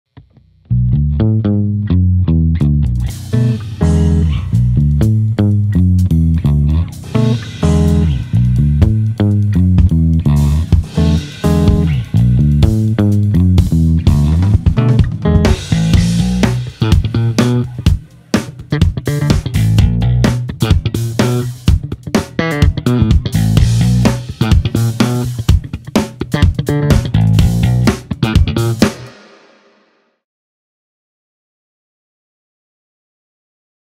Ernie Ball Music Man: Bongo 5 Demo with Mark Walker
From chords to slap in an instant.
Check out our friend Mark Walker on the new Mantis Green Bongo 5 HH.